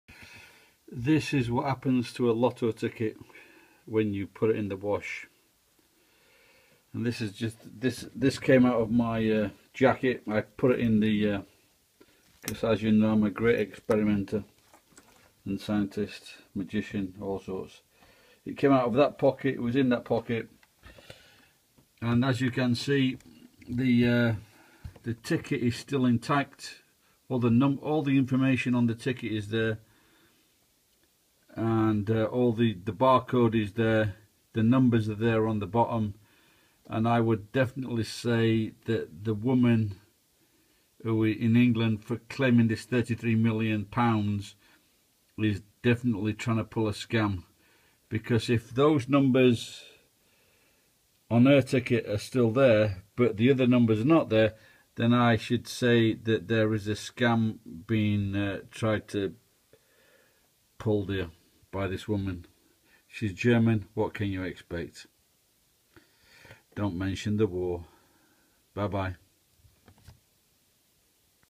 33 Million pounds UK loto scam experiment
After a 30 minute wash all of the ticket is still intact the numbers the bar code .I think this woamn is trying to pull a scam